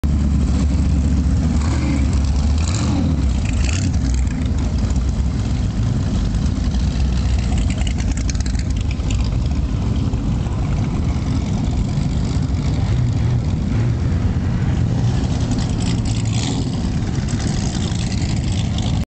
boss hoss daytona bike week 2010
Demo bikes at daytona.I rode the orange one quite a experience